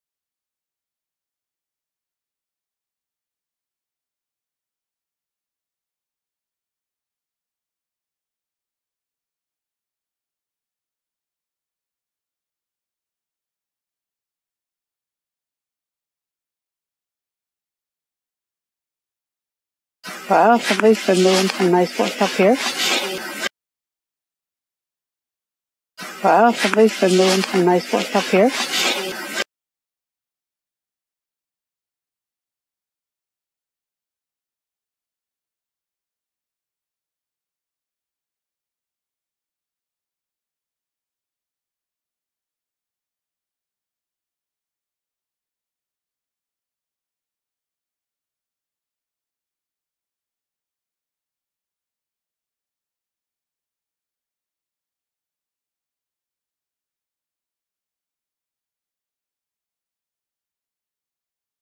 A haunted Caretaker in Cumberland County, NS
Spirits are always with us. This one decided to give us a tour.